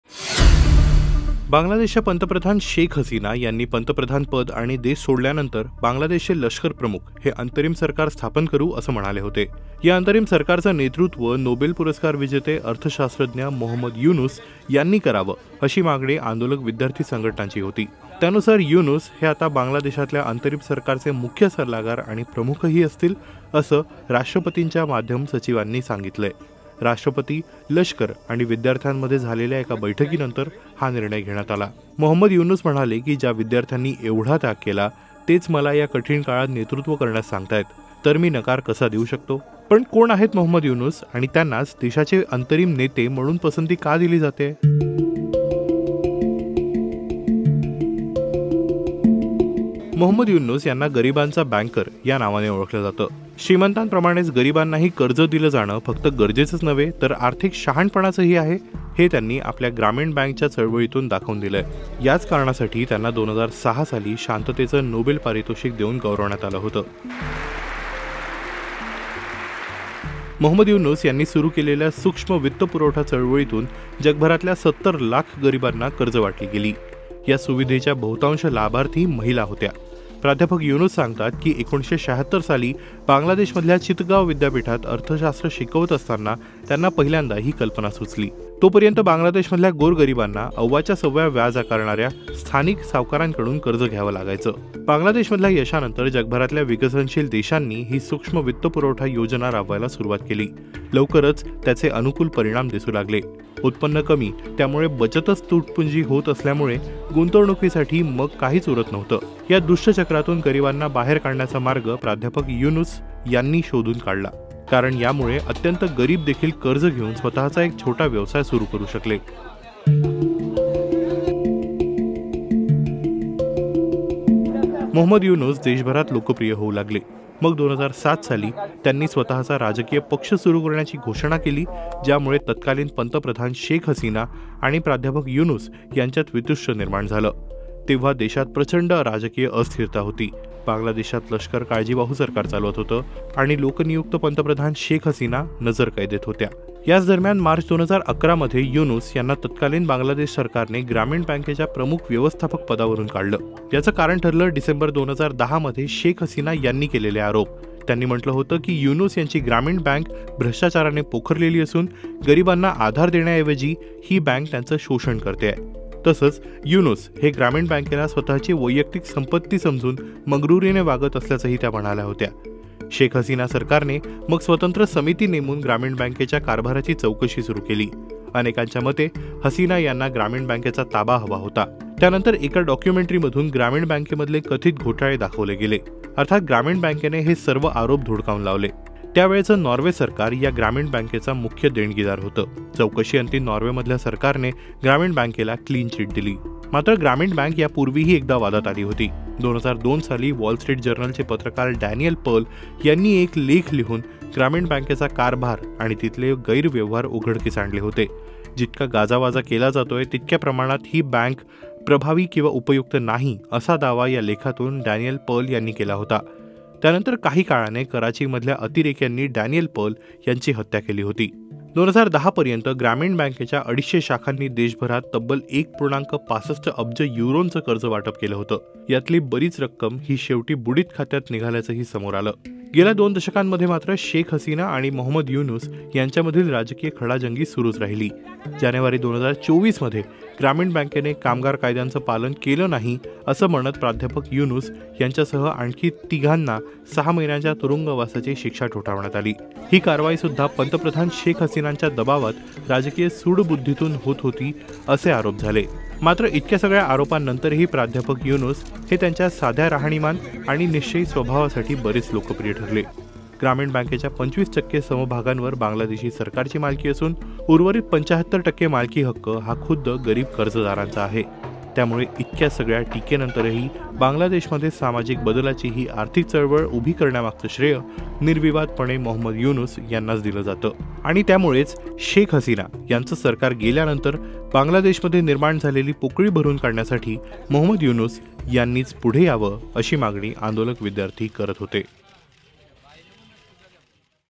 Muhammad Yunus Story: Sheikh Hasina यांच्यानंतर Bangladesh चे पंतप्रधान कोण?
बांगलादेशच्या पंतप्रधान शेख हसीना यांनी पंतप्रधानपद आणि देश सोडल्यानंतर बांगलादेशचे लष्करप्रमुख हे अंतरिम सरकार स्थापन करू, असं म्हणाले होते. या अंतरिम सरकारचं नेतृत्व नोबेल पारितोषिक विजेते अर्थशास्त्रज्ञ मोहम्मद युनूस यांनी करावं, अशी मागणी आंदोलक विद्यार्थी संघटनांची होती. त्यानुसार युनूस यांना बांगलादेशातील अंतरिम सरकारचे मुख्य सल्लागार आणि प्रमुख असतील, असं राष्ट्रपतींच्या माध्यम सचिवांनी सांगितलंय. राष्ट्रपती, लष्कर आणि विद्यार्थ्यांमध्ये झालेल्या बैठकीनंतर हा निर्णय घेण्यात आला.
मोहम्मद युनूस म्हणाले की, “ज्या विद्यार्थ्यांनी एवढा त्याग केला, तेच मला या कठीण काळात नेतृत्व करण्यास सांगत आहेत. तर मी नकार कसा देऊ शकतो.”
पण कोण आहेत मोह्म्मद युनूस आणि त्यांनाच देशाचे पुढचे नेते म्हणून पसंती का दिली जात होती?